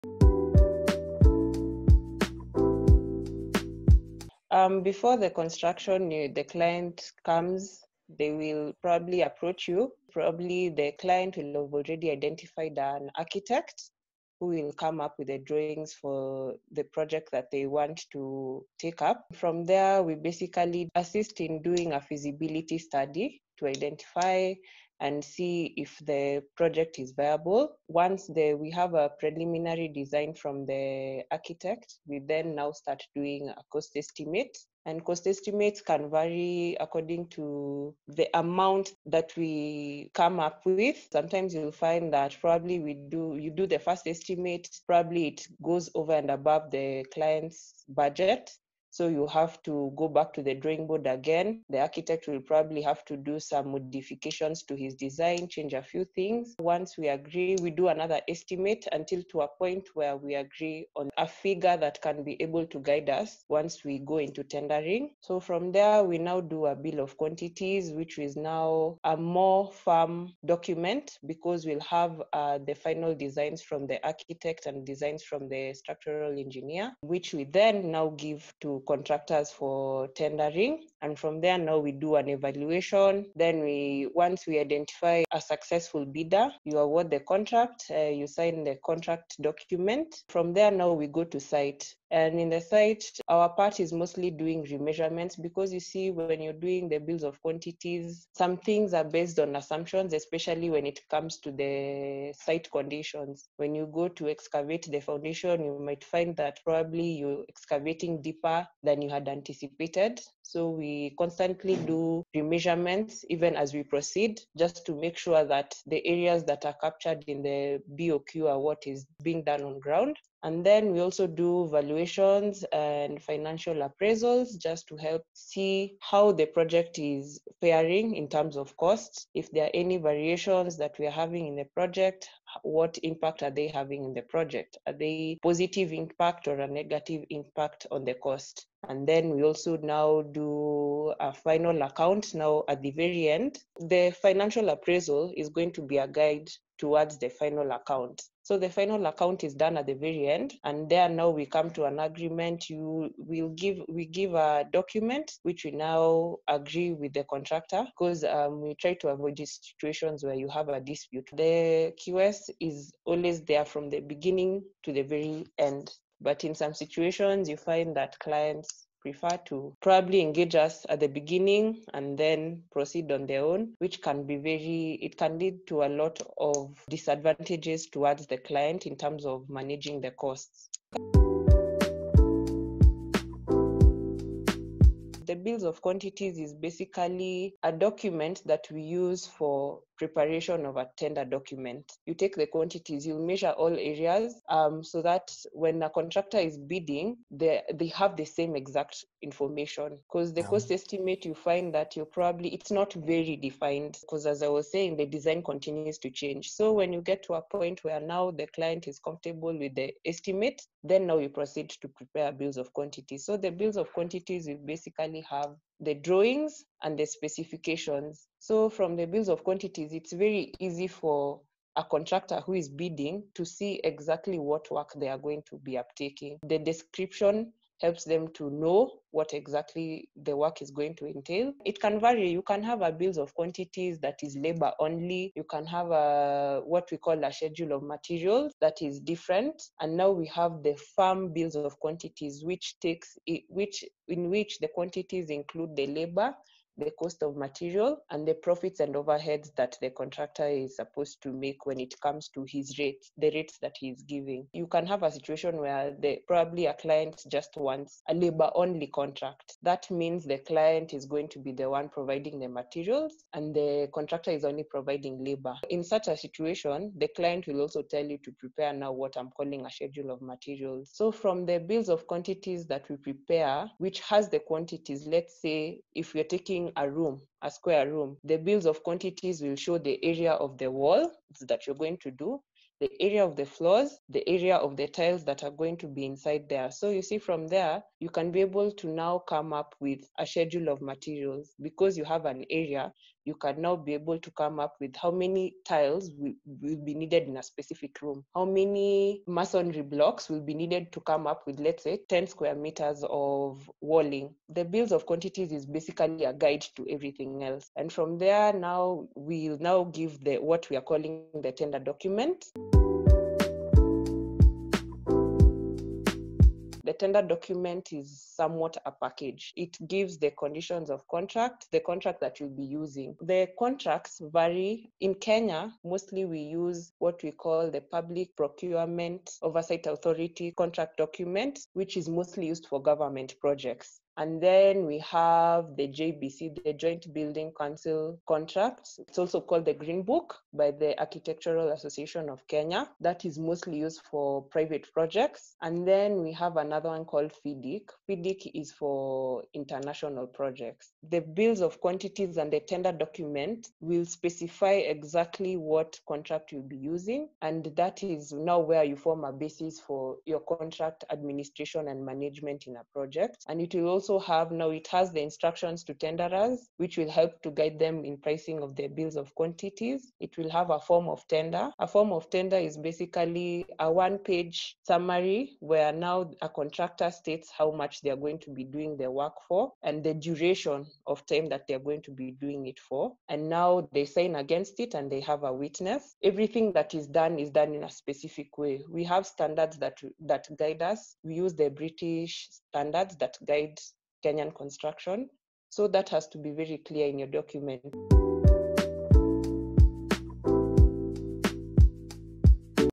Construction in Kenya: The Role of a Quantity Surveyor and the Bill of Quantities
This video covers the role of a quantity surveyor, the contents of the bill of quantities and the contents of the tender document in construction.

Speakers: Quantity Surveyor Joan Mbugua